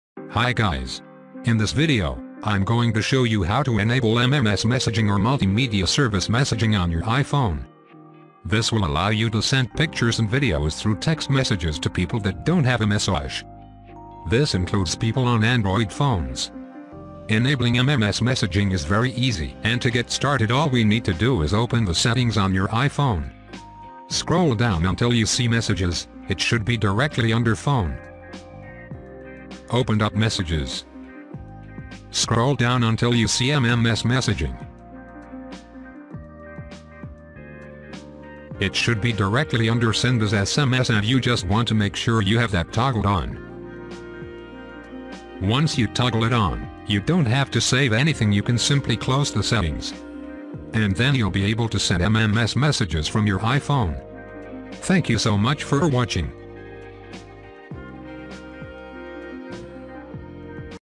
How To Enable MMS Messages On iPhone (2022)
In this video, I show you how to enable MMS messages on your iPhone. Enabling MMS messages will allow you to send videos, photos, and other media to people not using iMessage, such as Android users.
Here are the steps to turn on MMS messages on iPhone:
1. Open the settings.
2. Scroll down and select Messages.
3. Turn on MMS Messaging.
How to fix Cannot Send Message MMS Messaging needs to be enabled to send this message on iPhone

how to enable mms messages on iphone